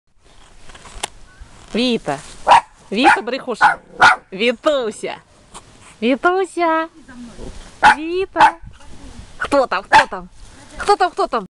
the Bichons at the cottage-3
Бишоны на даче.
Камелия Блум Виталина (Вита), 6 мес.
Вита - охранная собака.)))